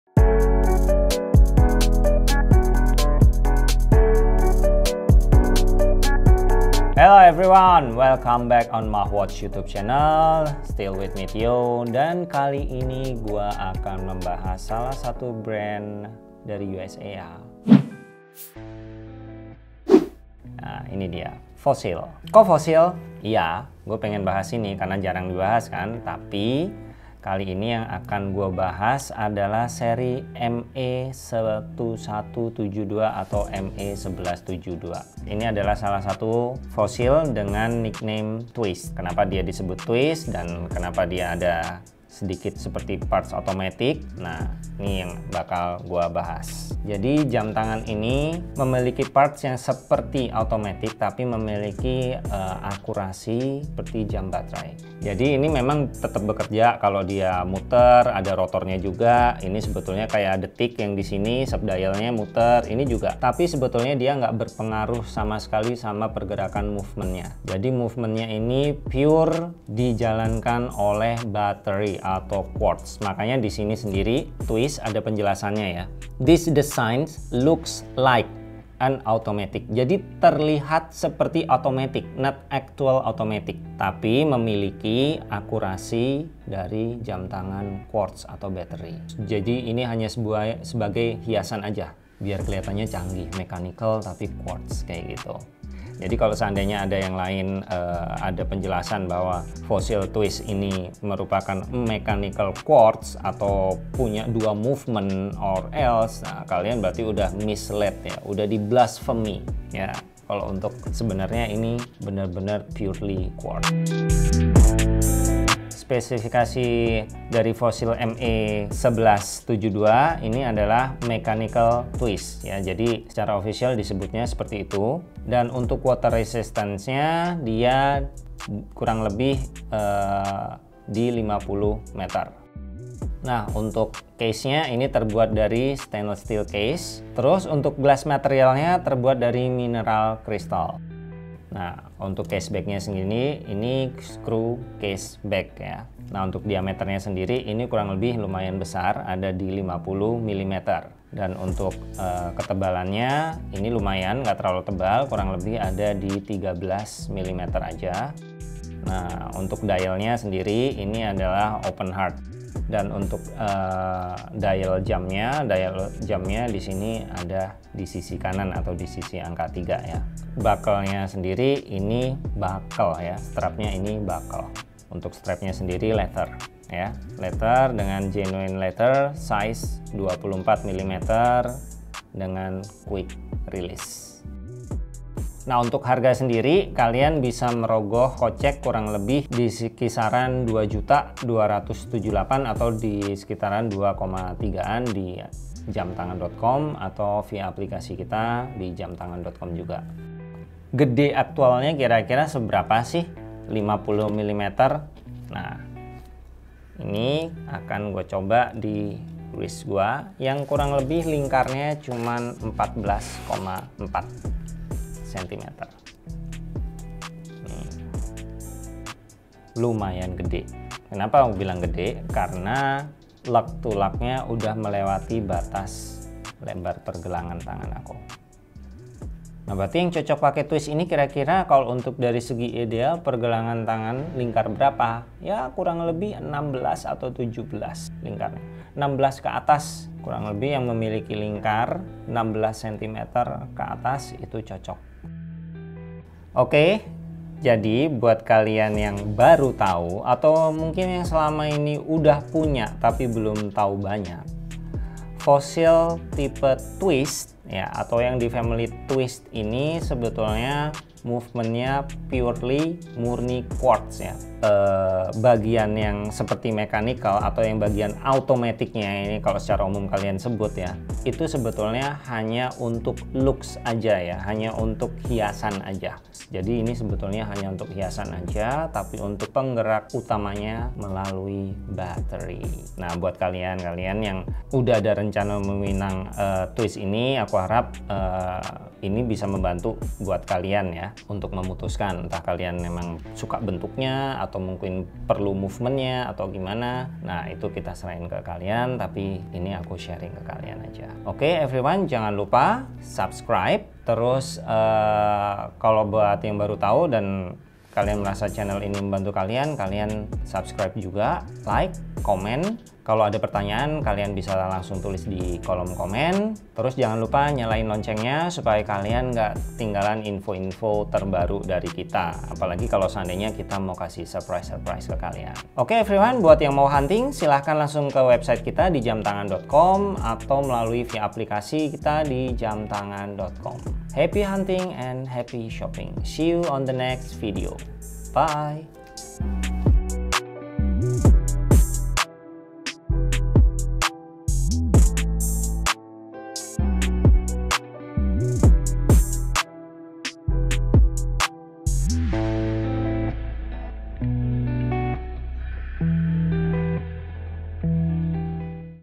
BANYAK YANG SALAH SANGKA! Di Balik Keindahan Dial Fossil Bronson Twist ME1172
Bukan sulap, bukan sihir! Pertama kali lihat Fossil Bronson Twist ME1172, mungkin mayoritas orang mikir itu jam tangan automatic. Gimana enggak, di dial ada komponen mirip gear hingga mainspring!

Tapi tunggu dulu guys, ternyata jam tangan itu butuh baterai lho. Jadi yang bener jam automatic atau quartz? Tonton sampe habis dan pecahkan plot twist-nya 😆

1. Fossil Bronson Twist ME1172